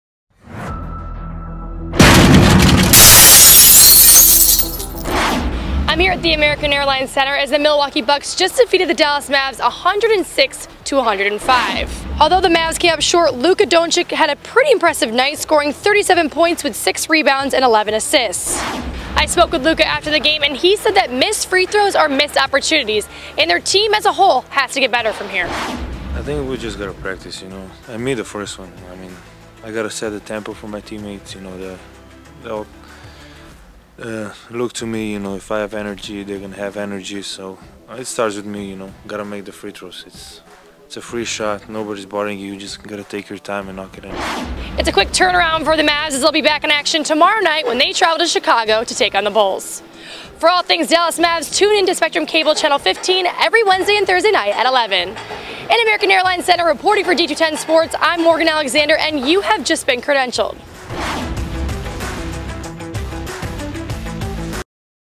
Dallas Mavericks Fall to the Milwaukee Bucks, 106-105 | Luka Dončić33 pts, 6 reb, 11 ast
The Dallas Mavericks' three-game win streak was snapped against the Milwaukee Bucks due to horrendous free-throw shooting down the stretch.